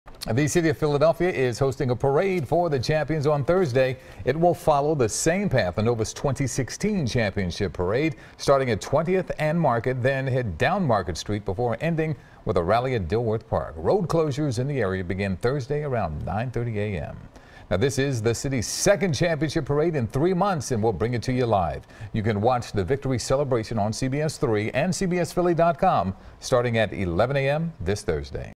Philadelphia To Host Villanova Championship Parade On Thursday
Philadelphia will be hosting its second championship parade in 2018 as the city will be celebrating Villanova’s NCAA title on Thursday.